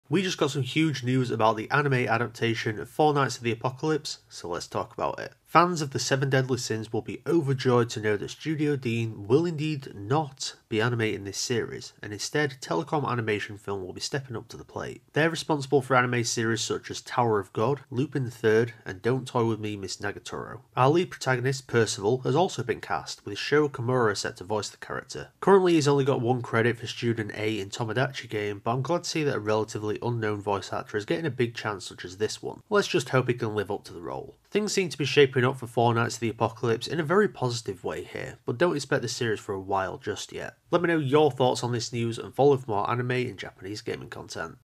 Four Knights of the Apocalypse | Anime Studio + Percival VA CONFIRMED!
The 'Four Knights of the Apocalypse' anime adaptation just got some huge news, so let's talk about it all!

XBL: MorphiusJR

PSN: mPlay711